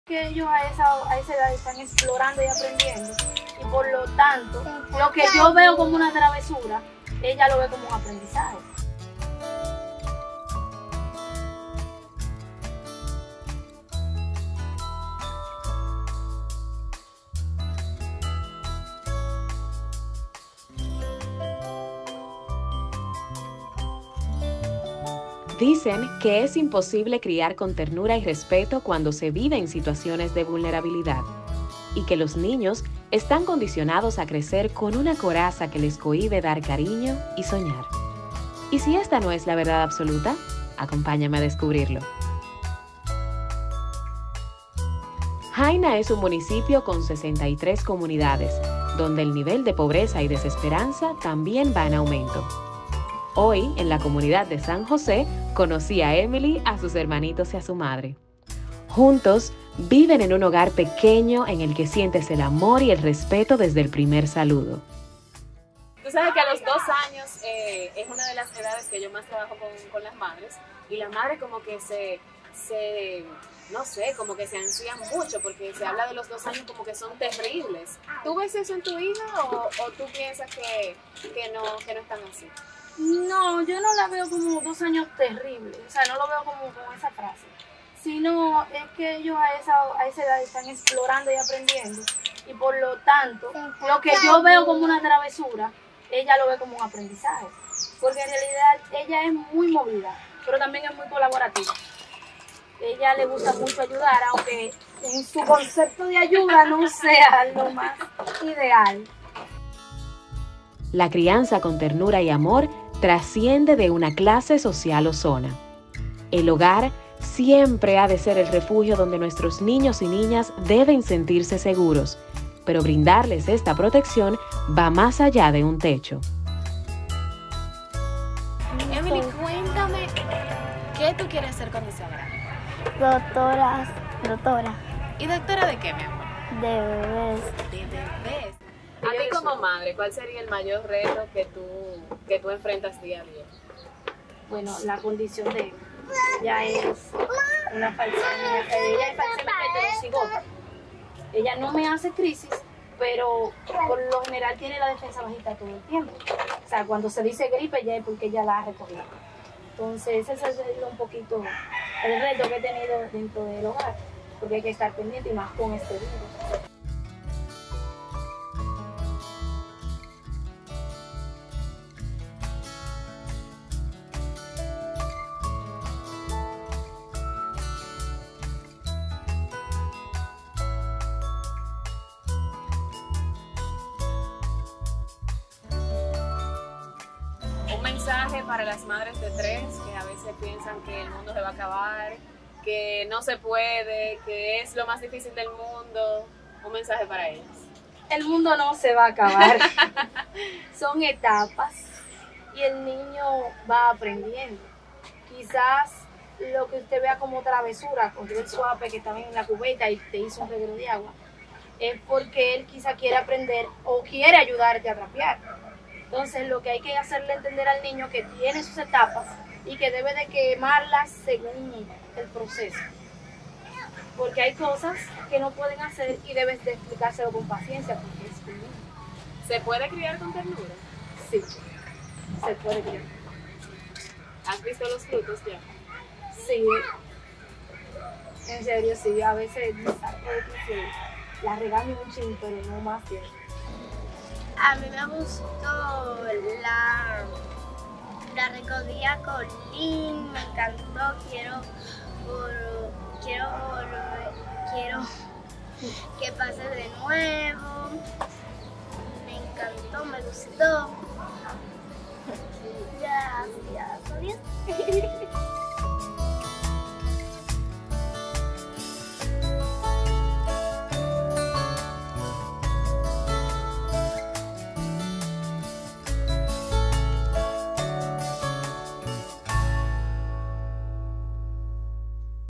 WVRD - Cuando eliges la Ternura para criar - AcompañaConmigo con Eileen Glass en Haina
Dicen que es imposible criar con ternura y respeto cuando se vive en situaciones de vulnerabilidad.
Y que los niños están condicionados a crecer con una coraza que les cohíbe dar cariño y soñar. ¿Y si esta no es la verdad absoluta?

Descubre cómo puedes ayudar en el compromiso de las comunidades más vulnerables de nuestro país para capacitarse, crecer y transformarse con Ternura en:

¡Necesitamos el compromiso de todos!